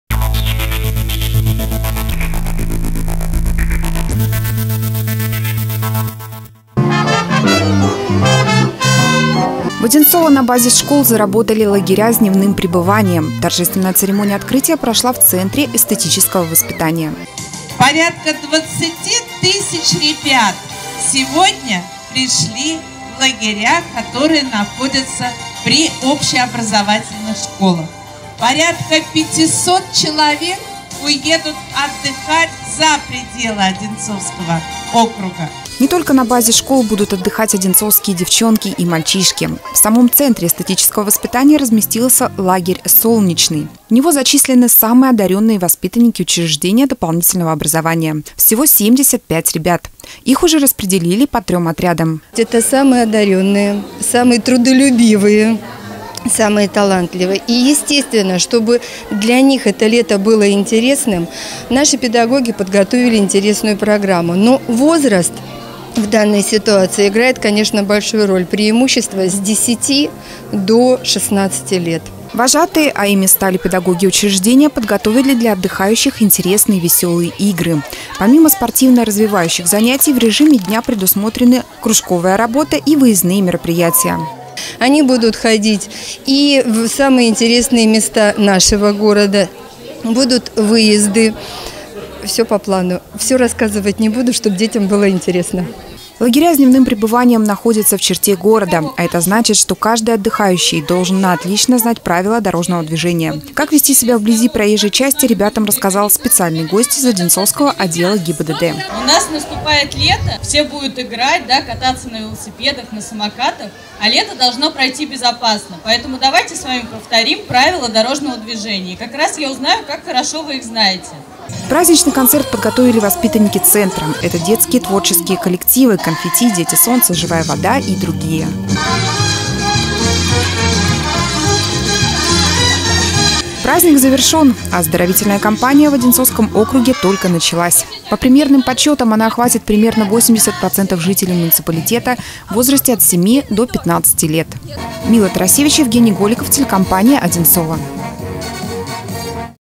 Открытие лагерной смены в ОЦЭВ
На базе Одинцовского центра эстетического воспитания открылся лагерь «Солнечный». На торжественной церемонии выступили воспитанники учреждений культуры.